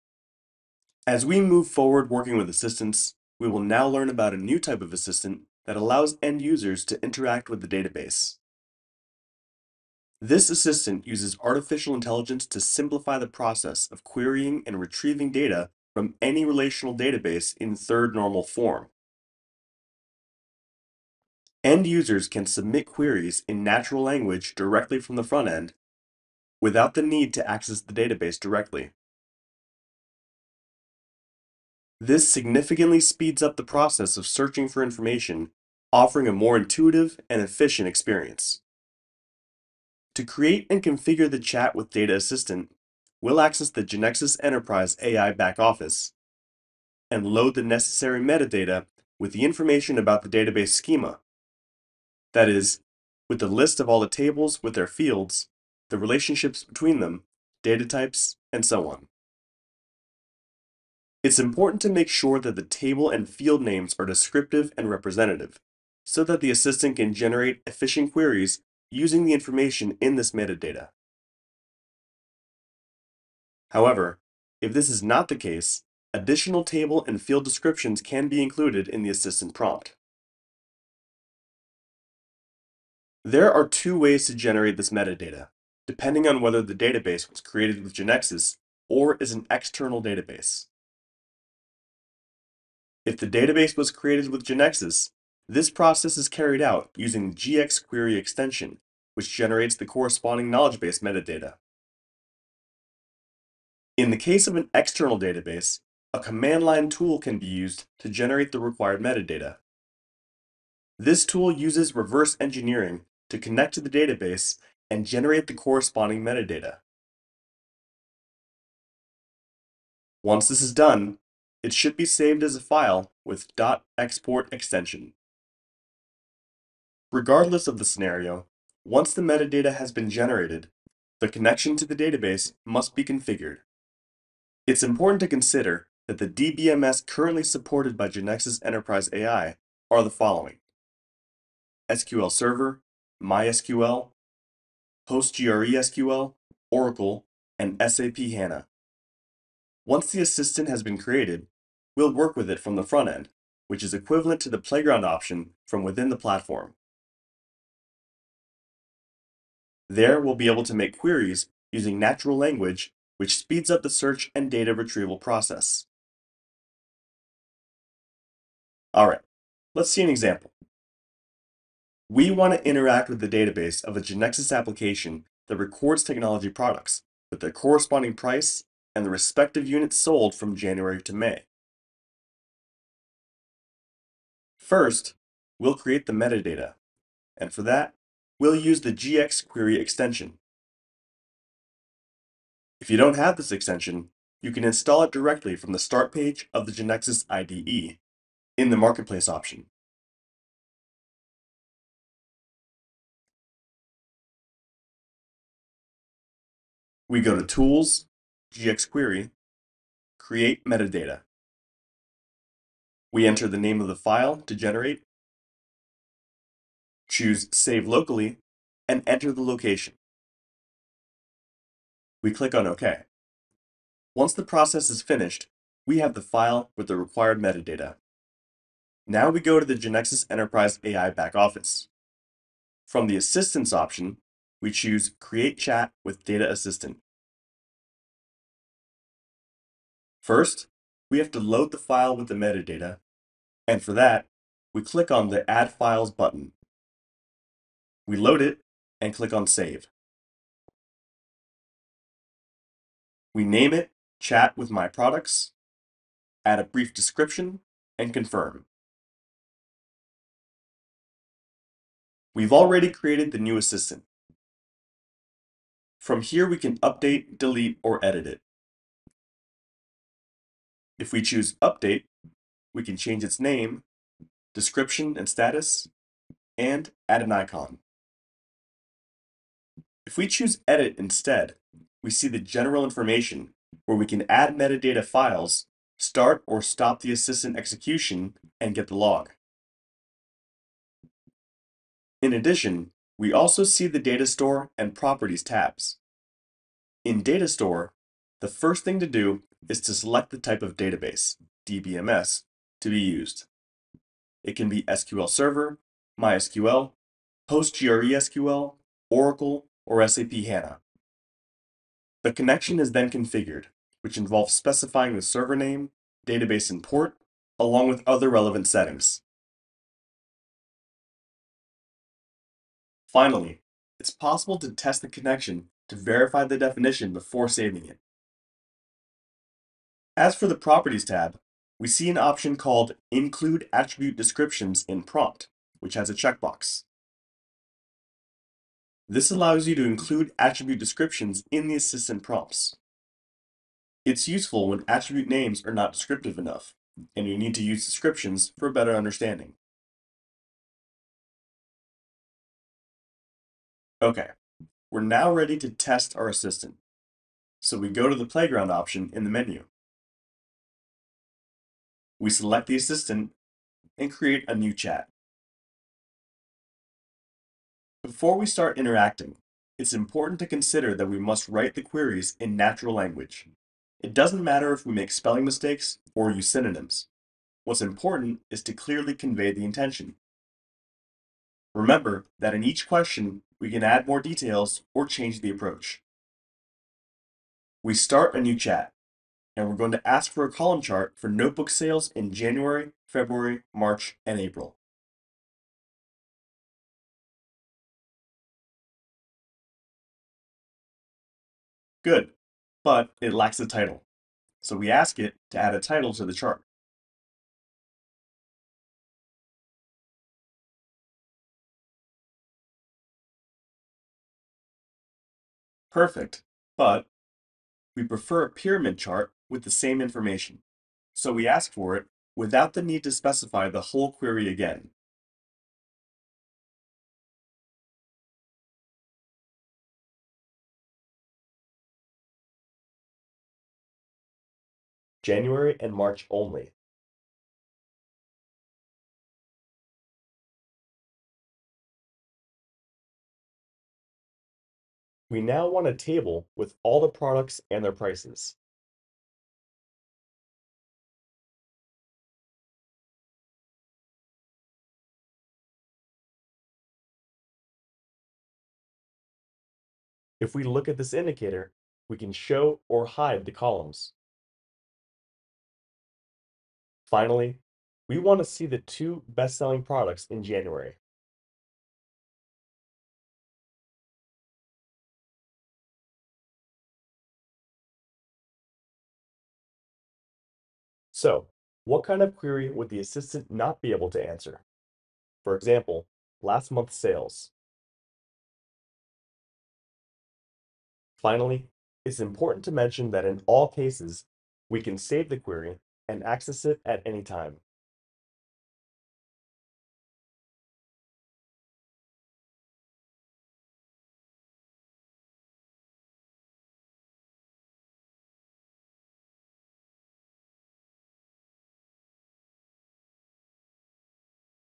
Chat with Data Assistant. Creation and use
We learn about a new type of assistant that will allow the end user to interact with the database. This assistant uses artificial intelligence to simplify the process of querying and retrieving data from any relational database in third normal form.

End users can submit queries in natural language directly from the Frontend without the need to access the database directly.